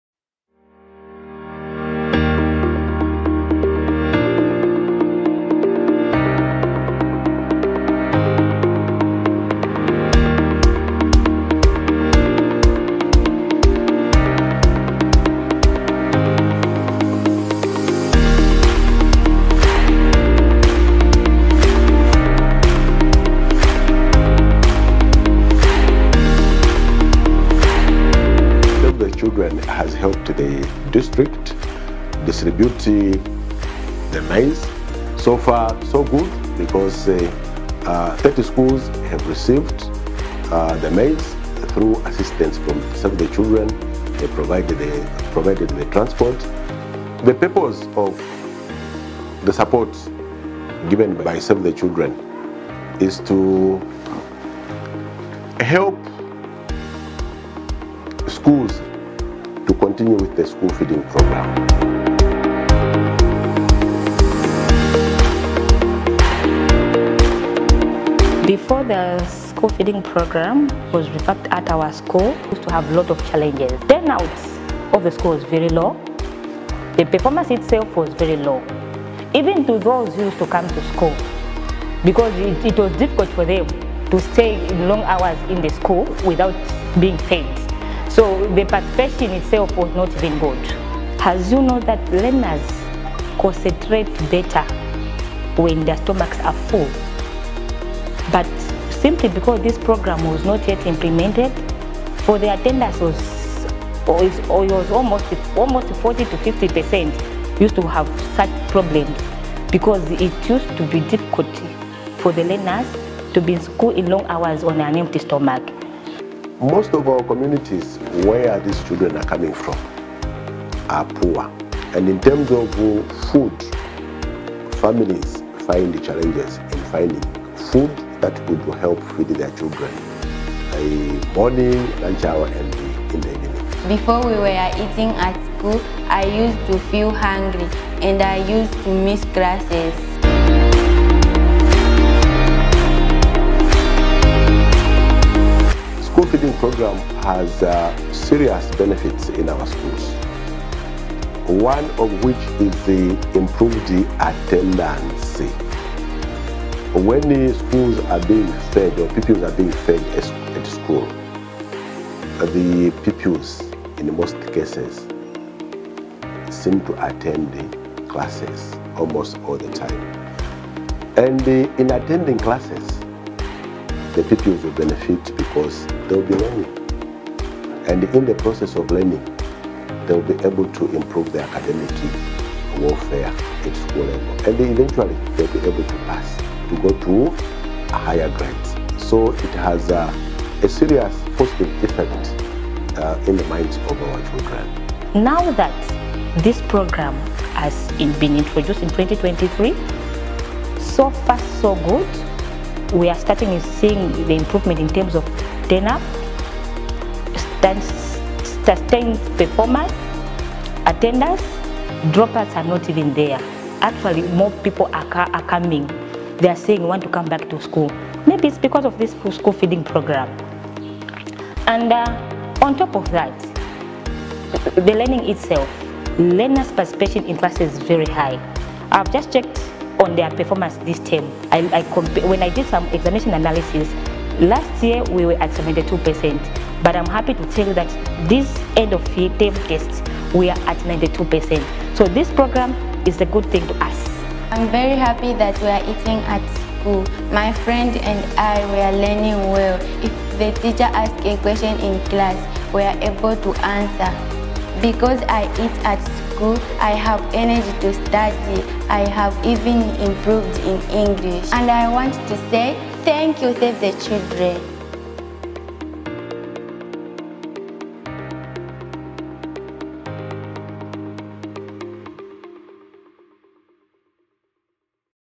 Flood Response School Feeding program
We are proud to have collaborated with the Kalabo
District Education Board to deliver 115 Metric tonnes of maize grain
to 147 schools, ensuring that children like Lubeta have access to hot
meals through the school feeding programme.